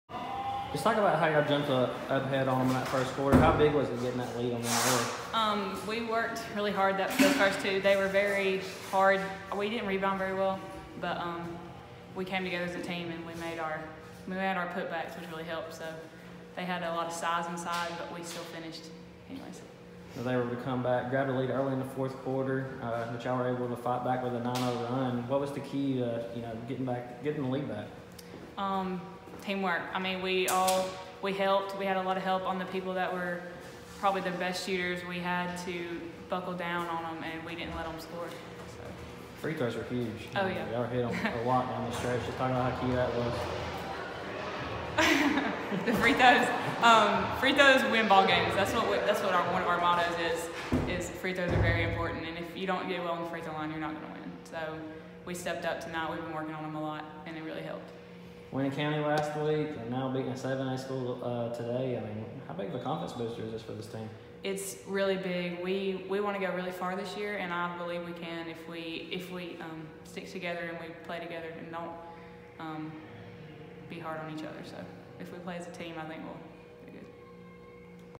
Glencoe's Lauren Cole talks 60-51 win over Gadsden City
Glencoe's Lauren Cole scores 14 PTS in the Yellow Jackets' 60-51 win over Gadsden City on Jan. 22.